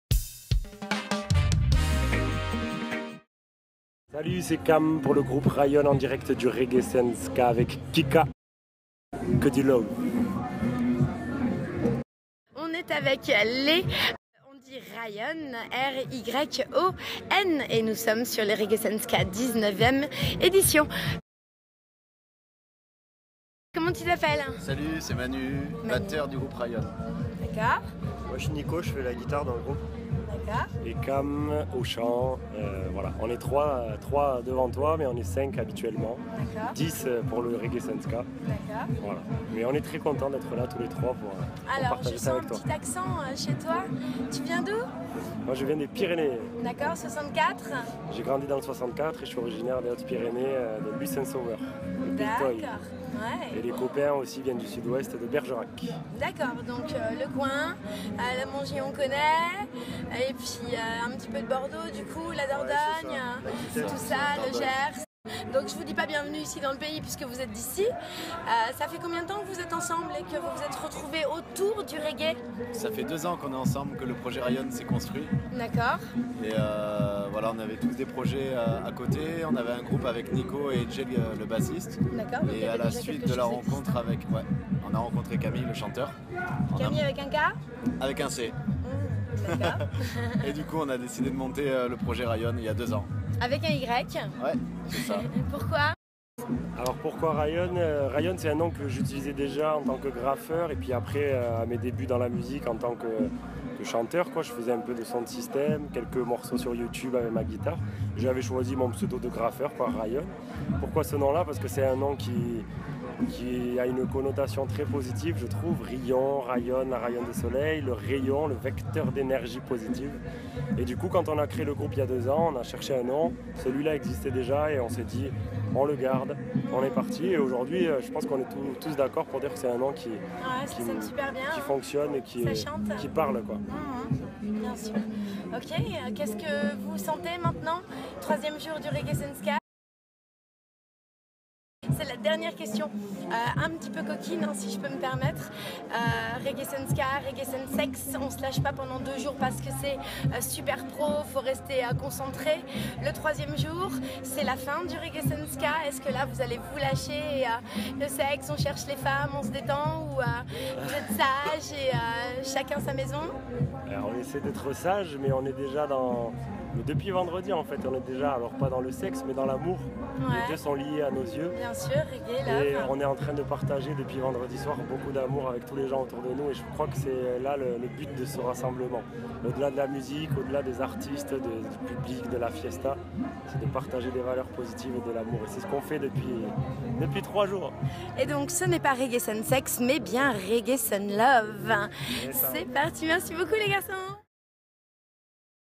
ITW du groupe @Ryon by KIKA
Première prise de contact avec le groupe RYON qui se présente, nous parle de leurs origines, du pourquoi RYON et ils répondent à la question coquine ;)